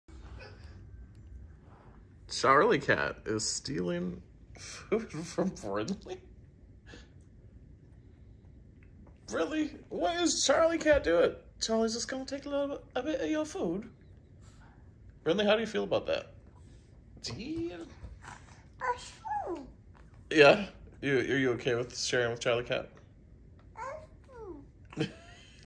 Like taking candy from a baby.
Charlie cat steals from a baby!